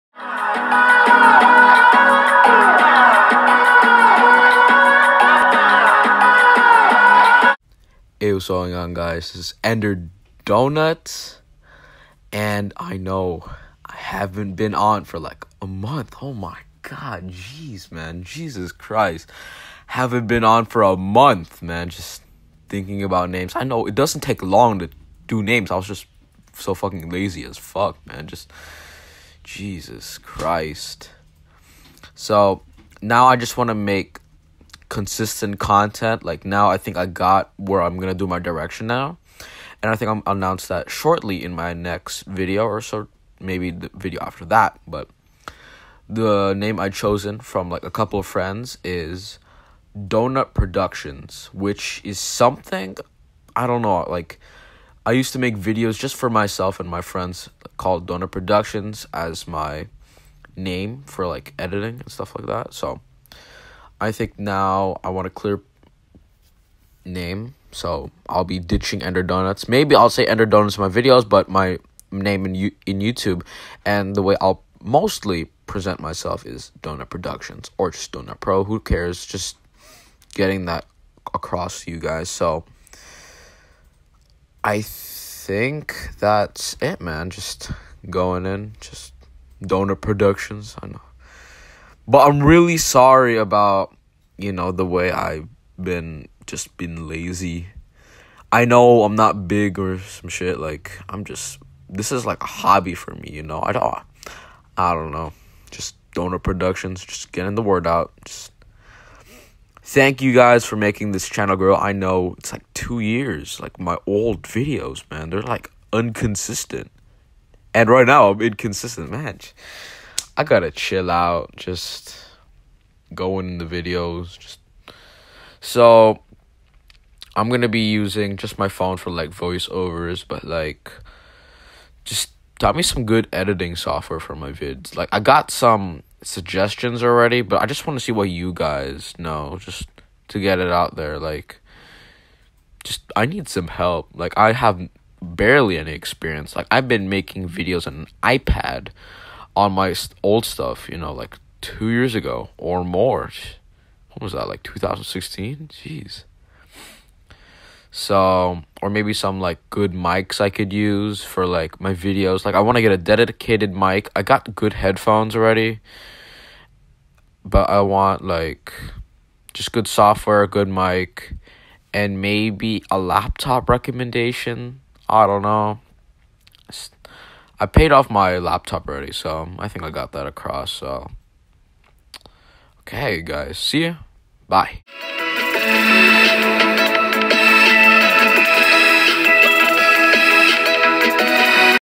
Name change finally
Yo sorry for the unnecessary wait time and I know it took too long. Changed the gameplay because I got a little tired of fortnite.